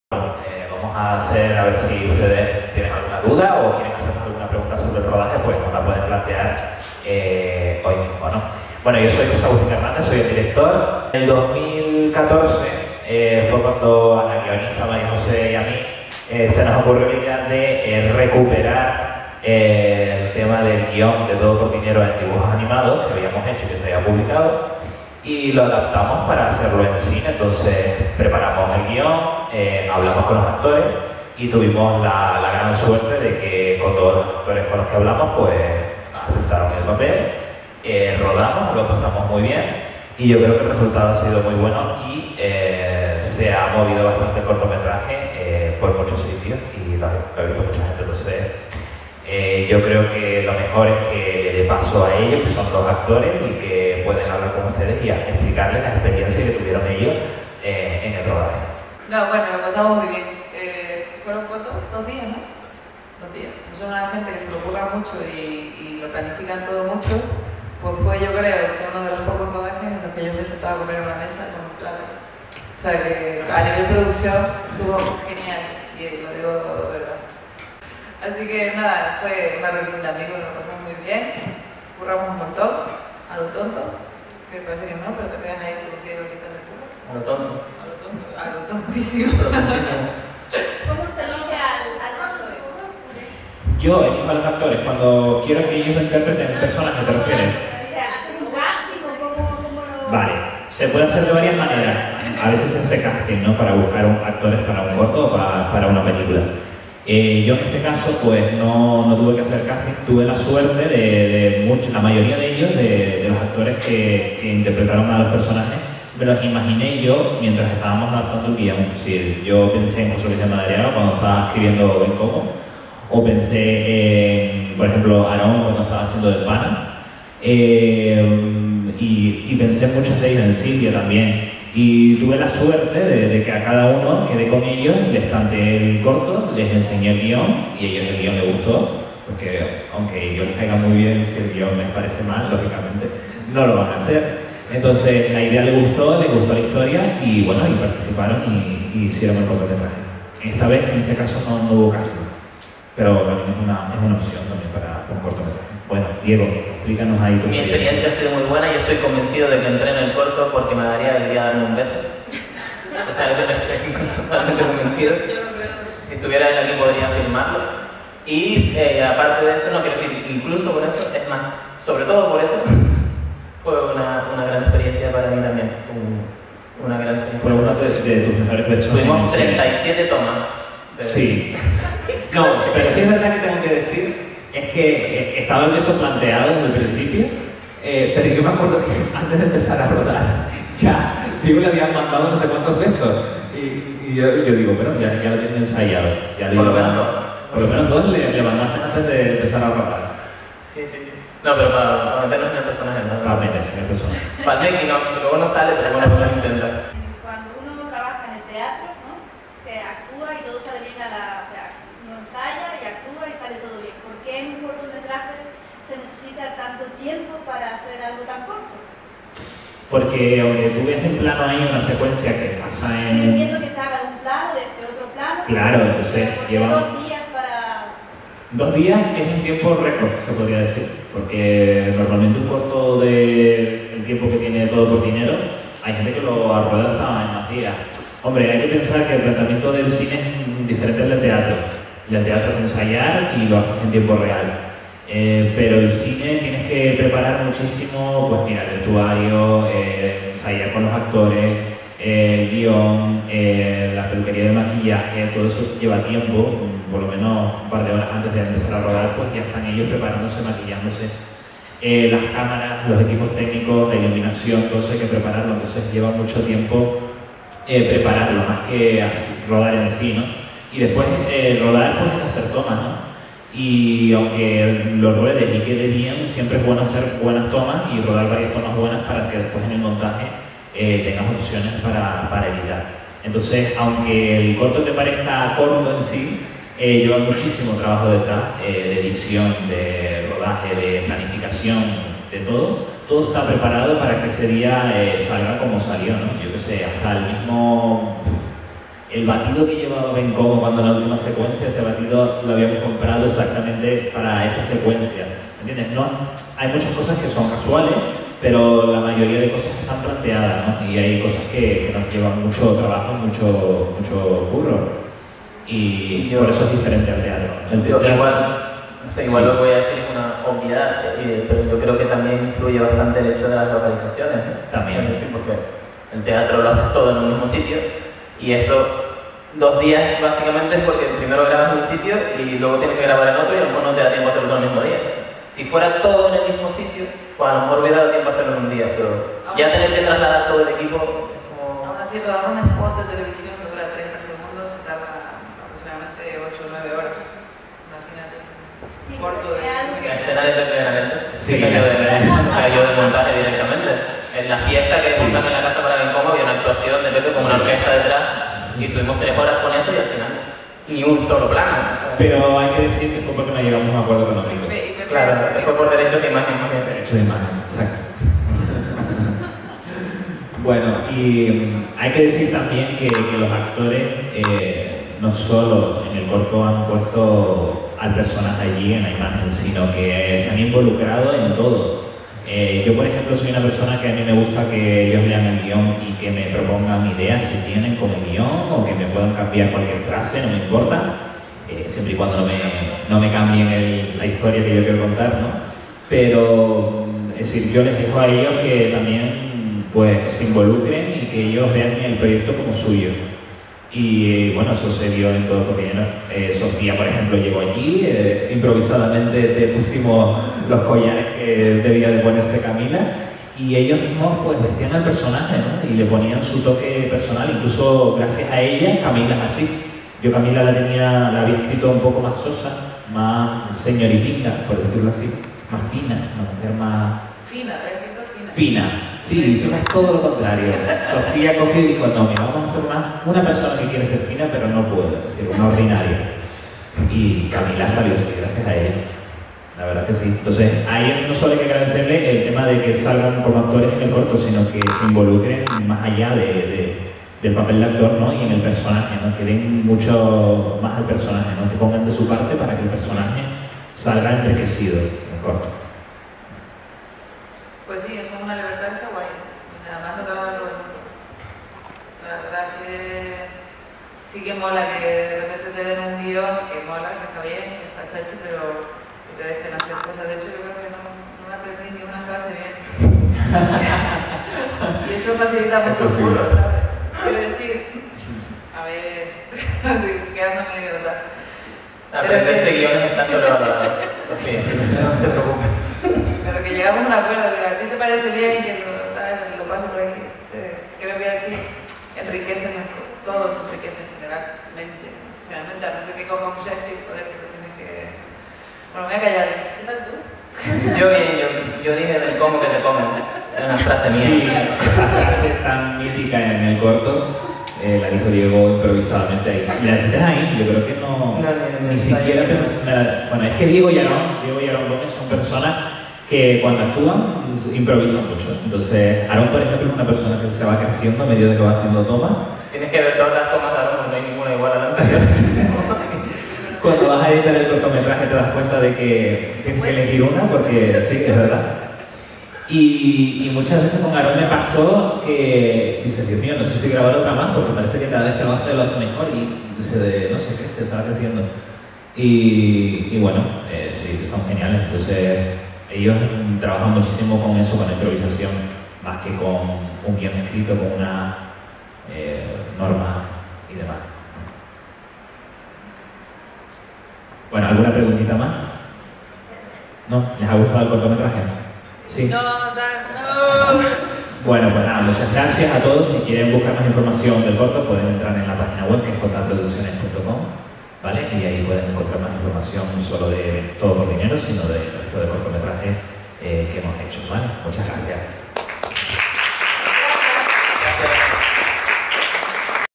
Todo por Dinero - Proyección TLP Tenerife - J.A. Producciones © 2015
Charla con los actores Sofía Privitera y Diego Lupiáñez junto al director José Agustín Hdez. con motivo de la proyección de "Todo por Dinero" en Tenerife Lan Party (TLP Tenerife 2015) en el Recinto Ferial de Tenerife el 17 de Junio.